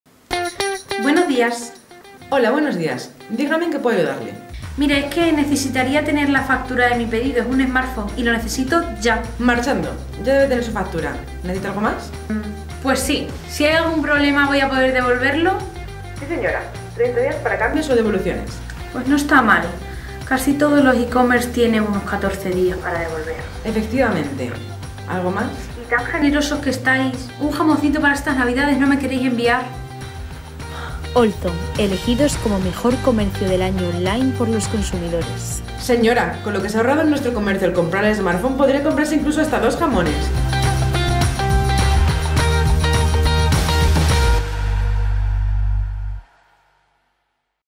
Rapidez y eficacia con cada cliente | Departamentos de FACTURACIÓN y ATENCIÓN AL CLIENTE
Si te dedicas al mundo del ecommerce sabrás de lo que hablamos... ¡Los clientes piden, piden y piden!

En Allzone queremos consentiros siempre que esté en nuestra mano con las mejores ofertas del mercado y productos de altísima calidad. Por supuesto, acompañado de la proximidad total con el cliente, tanto para gestionar su factura como para resolver cualquiera de sus dudas con la garantía o las devoluciones.

¡Recuerda! Podrás gestionar tanto la garantía como la devolución de un pedido desde el área de cliente de nuestra página web.